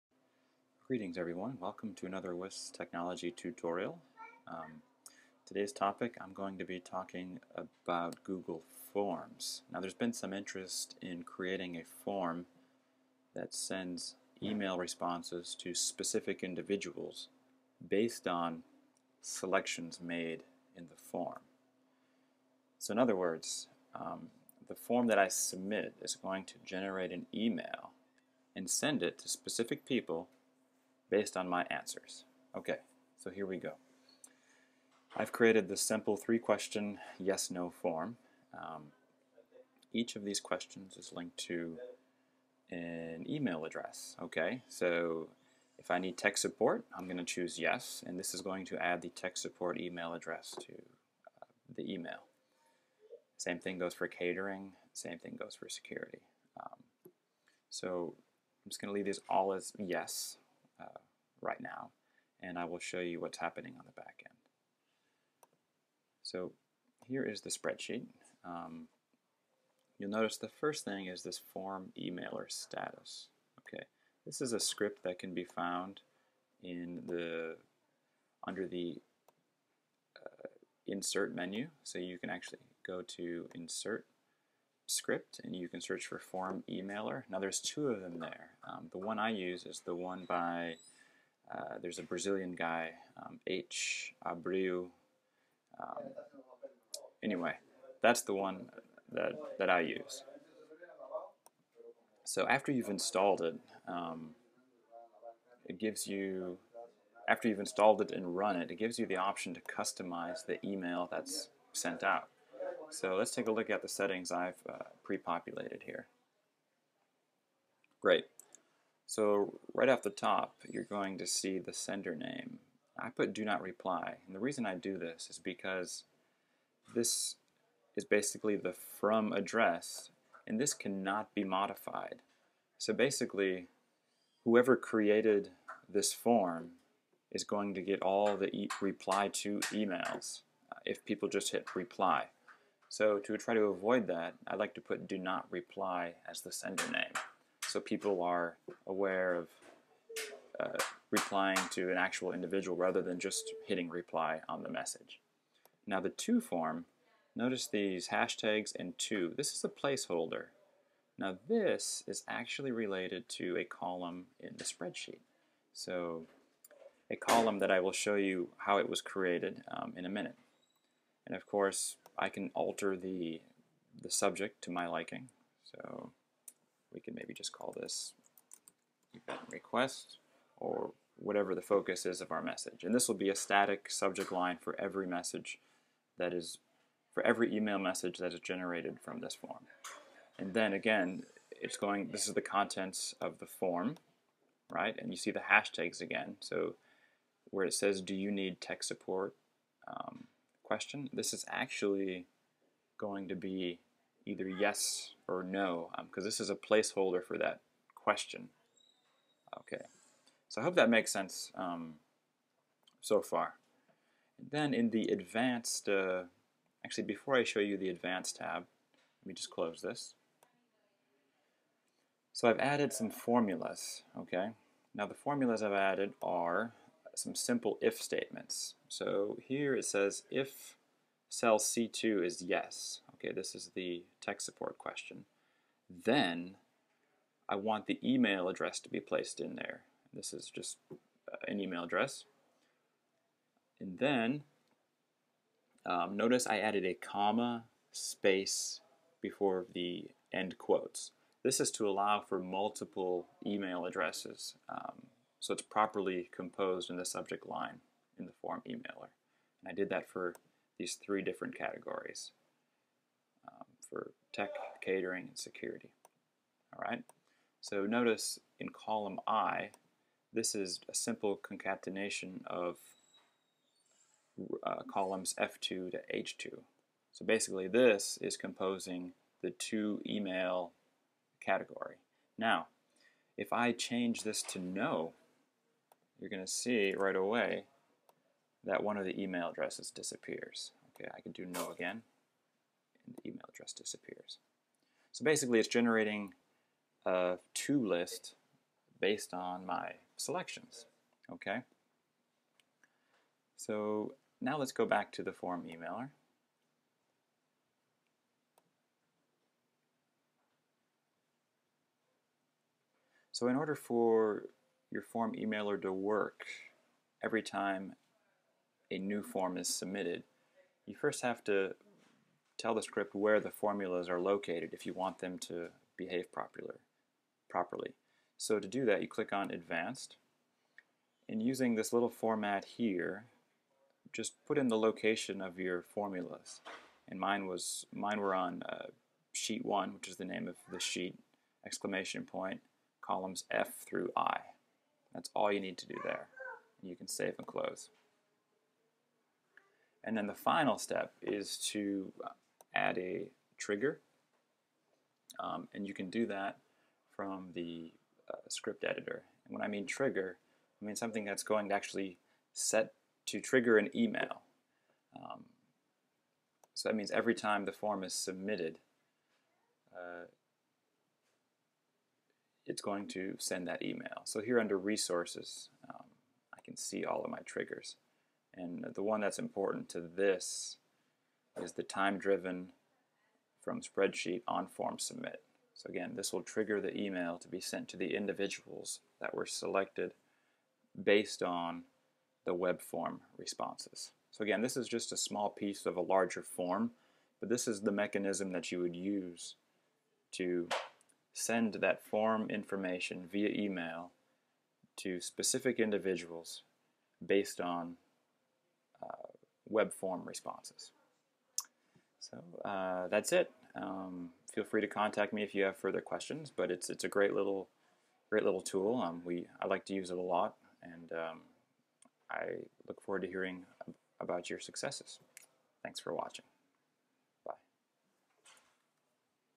FormEmailer: Sending emails to multiple accounts based on webform responses.
This tutorial shows you how to build a webform that automatically sends an email to people based on the users webform responses.